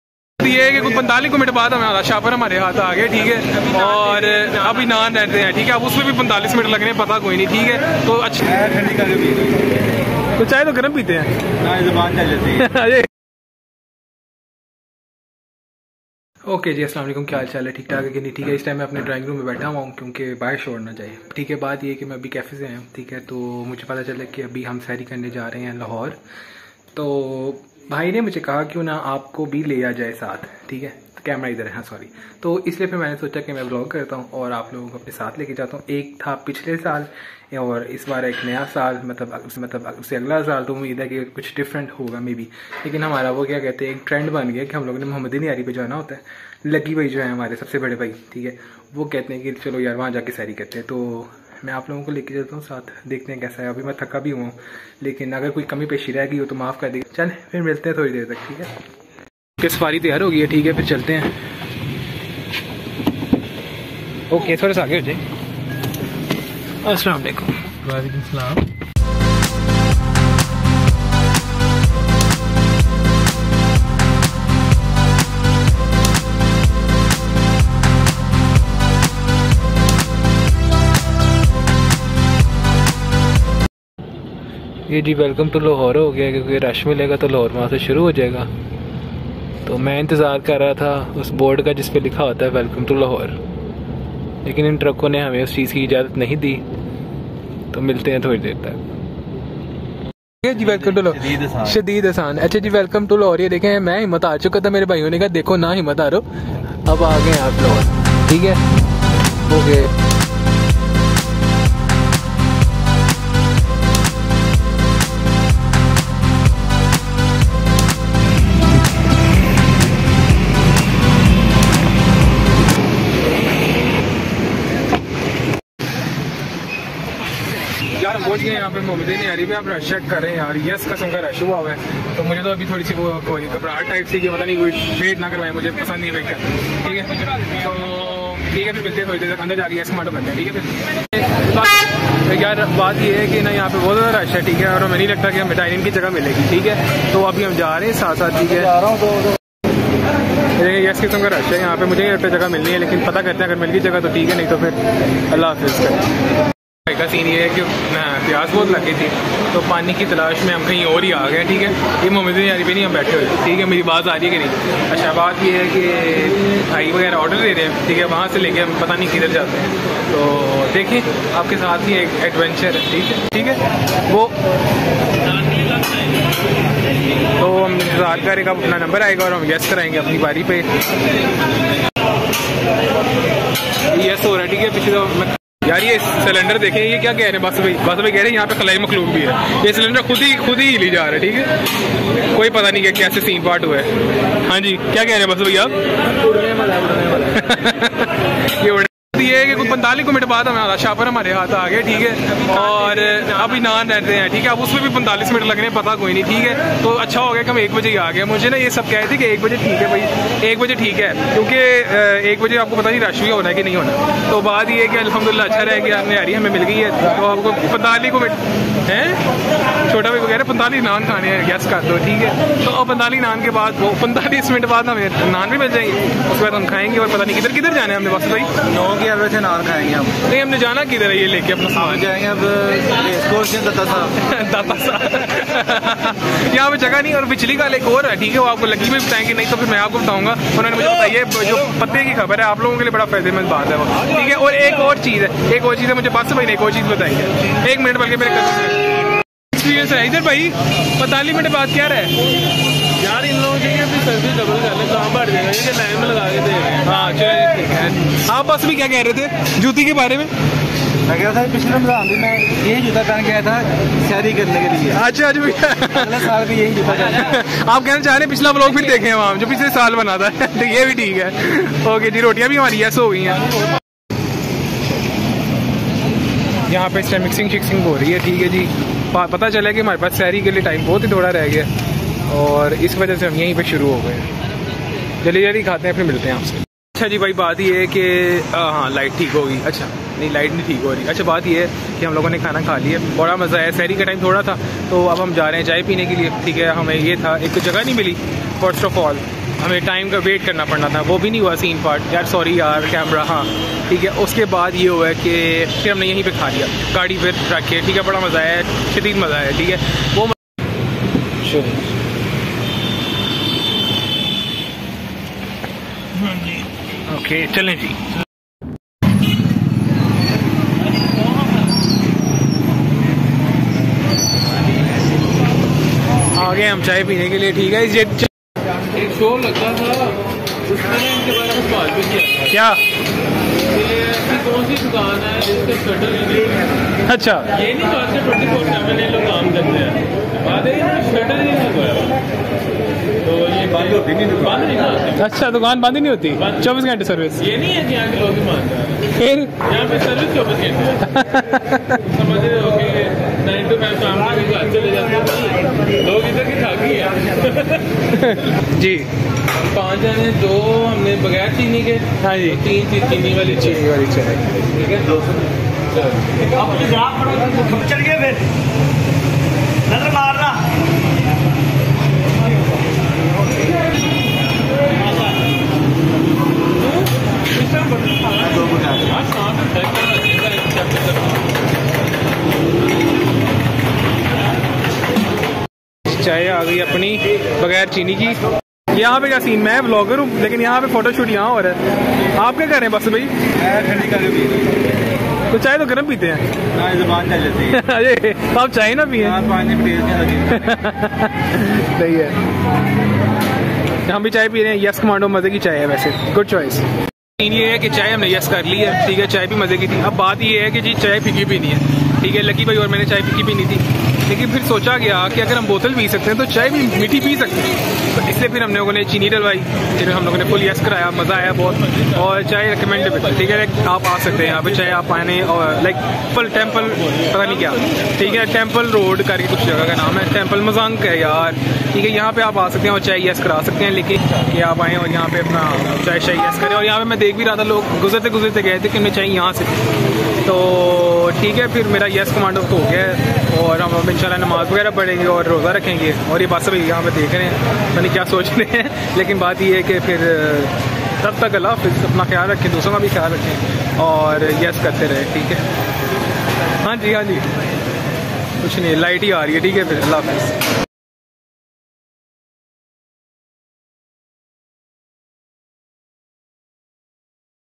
Boys Night out. we went to Lahore for sehri .we saw a gin 😂
we all boys went to muhammadi nehari house for sehri and we did get the place